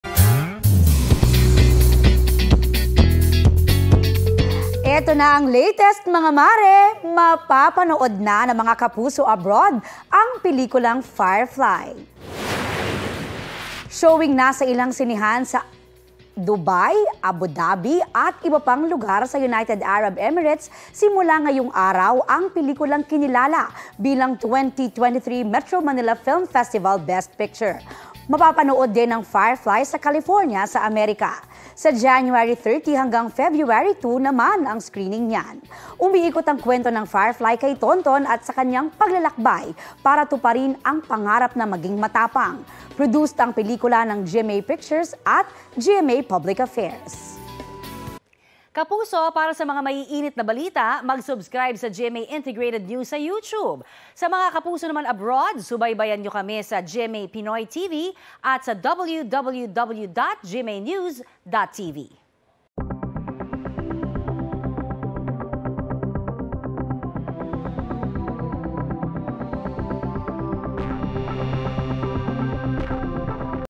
2023 MMFF Best Picture "Firefly," mapapanood na sa ilang sinehan sa U.A.E. simula ngayong araw | BT
Mapapanood na ng mga Kapuso abroad ang pelikulang "Firefly."